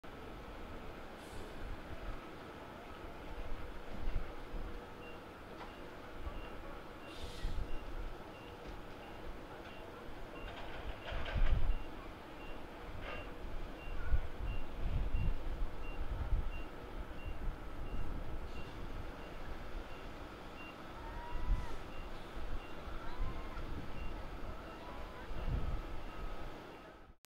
Kayaking and Pelicans at the Pier
Pelicans are at the edge of the fishing boat, while kayakers go by.